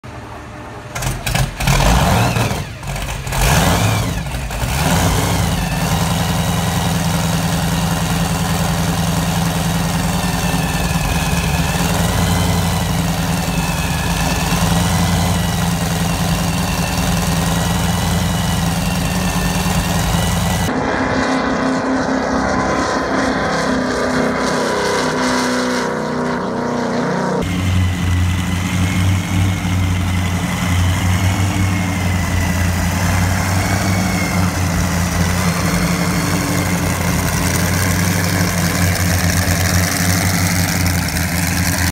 Kenny Mungle and John Tomlinson of TNT Custom Marine tested Mungle's 36-foot Skater in Miami today.
Kenny Mungle and John Tomlinson of TNT Custom Marine tested Mungle's 36-foot Skater in Miami on Monday.